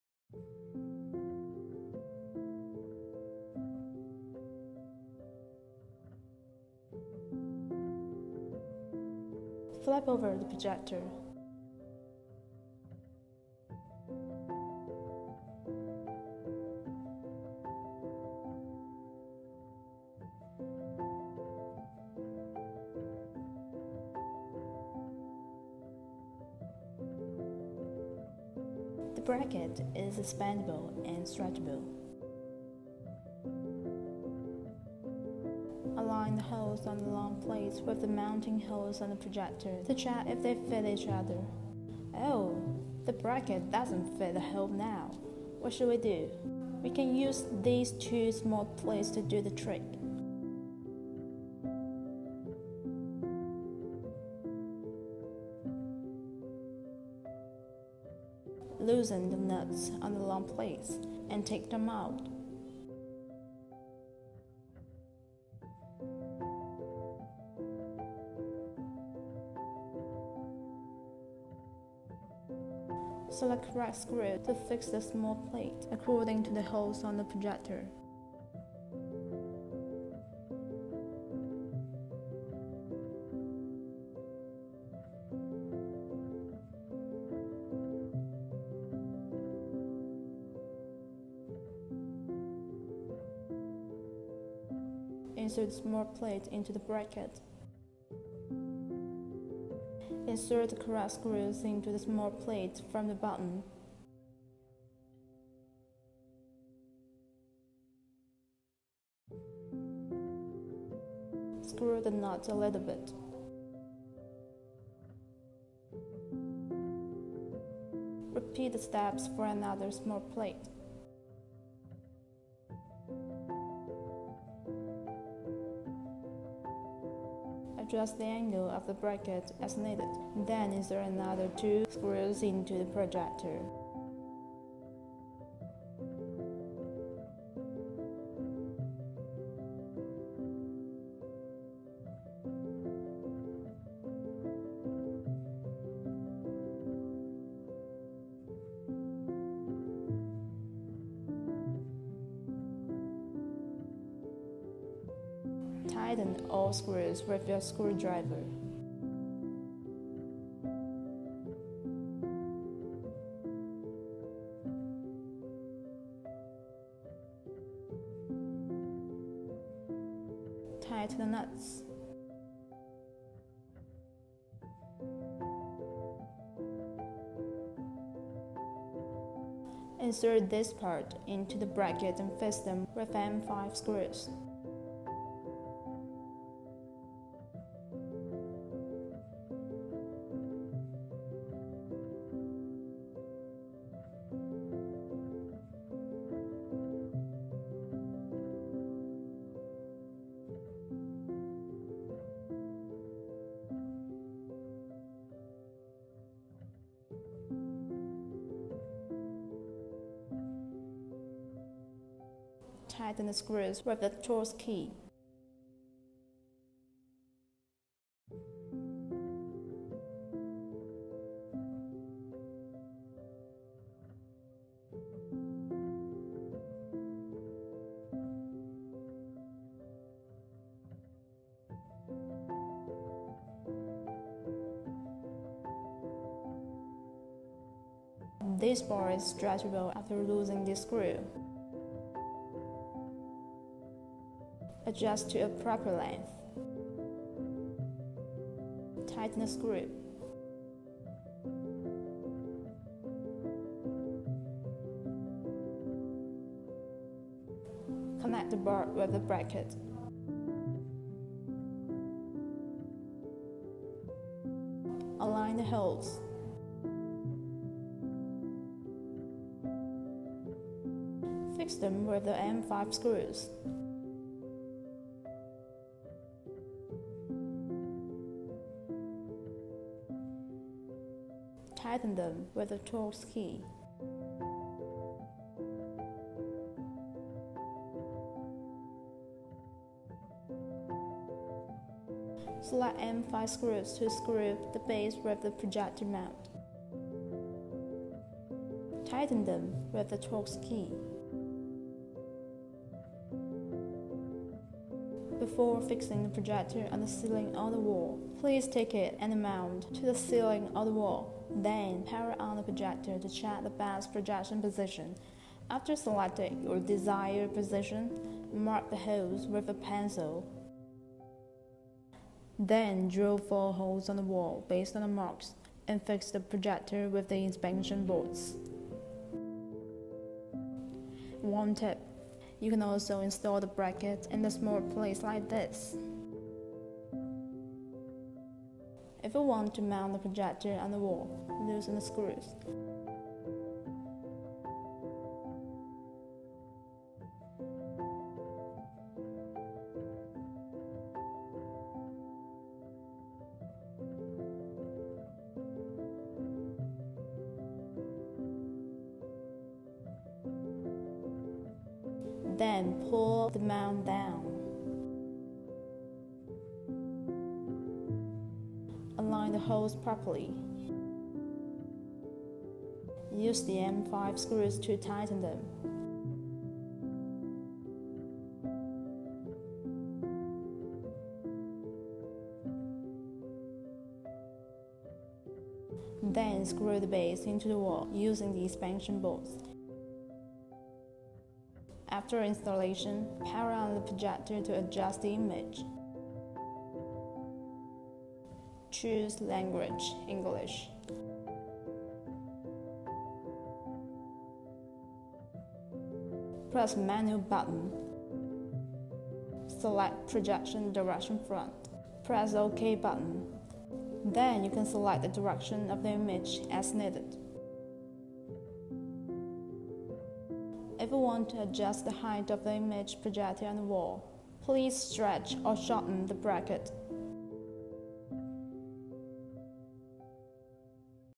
Installation Guide for VANKYO Projector Mount PM20
It is better to install the two small plates into two horizontally aligned screw holes, as the picture during 05:31-05:37 shows.